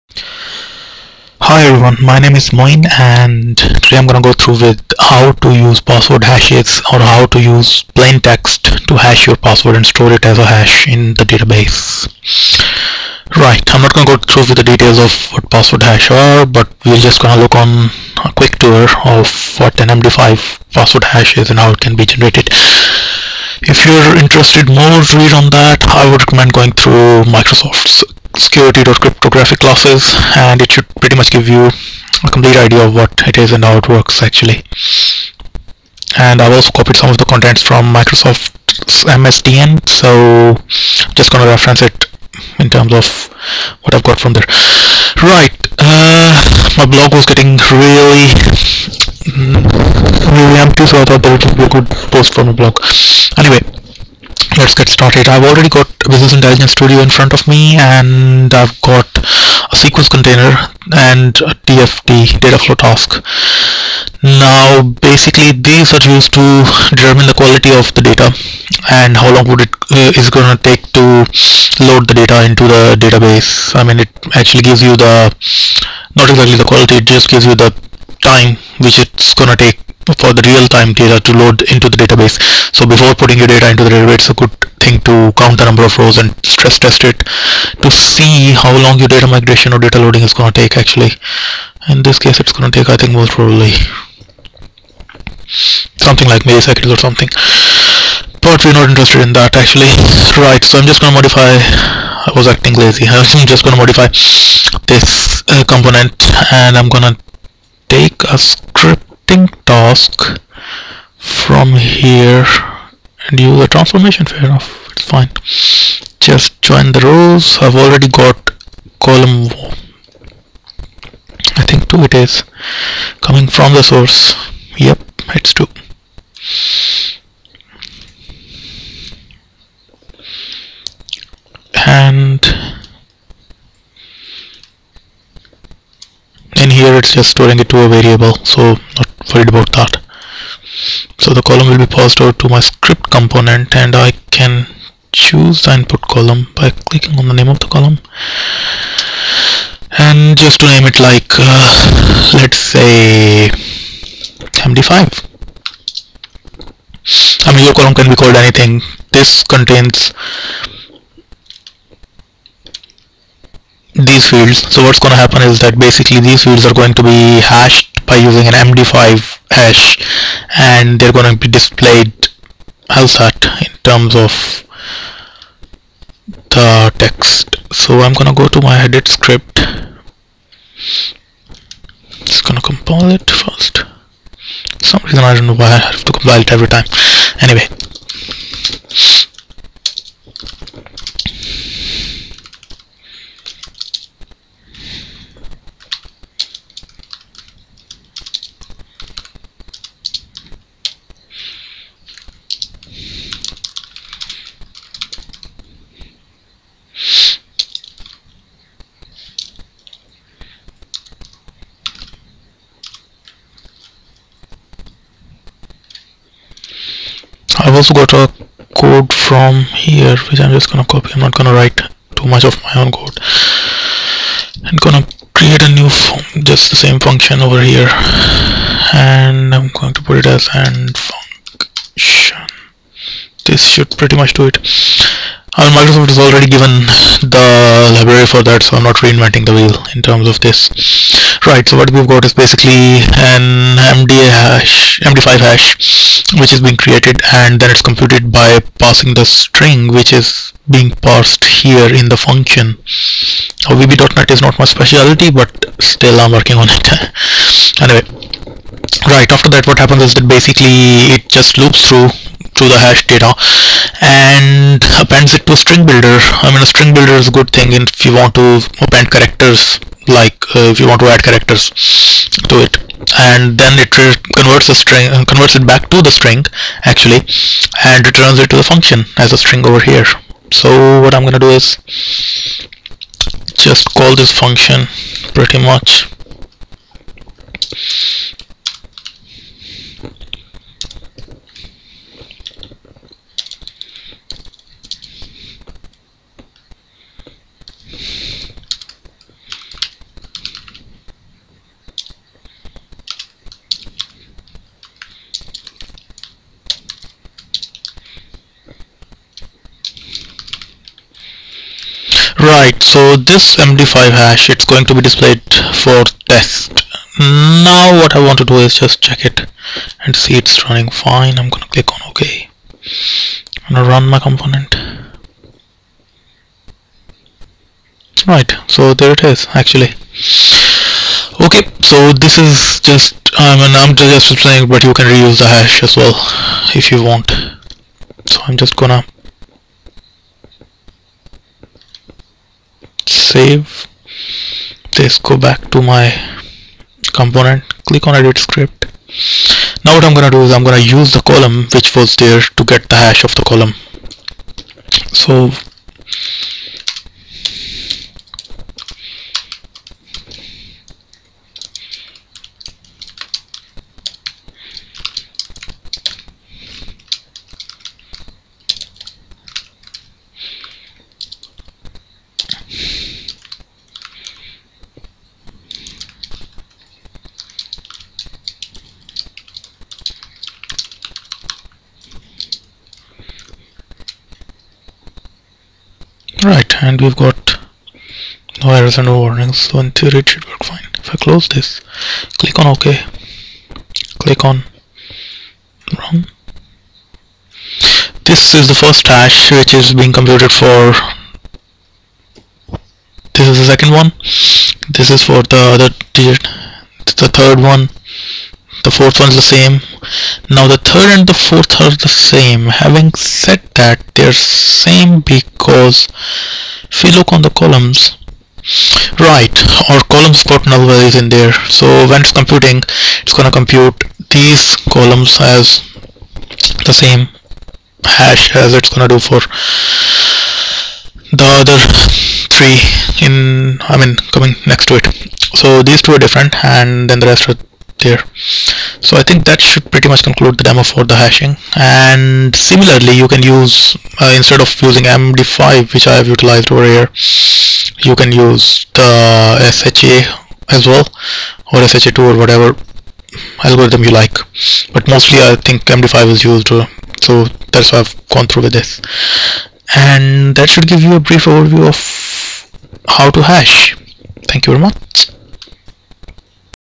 Generate MD5 Hash from Text String for SSIS
how to generate MD5 hash from text string to use in ssis.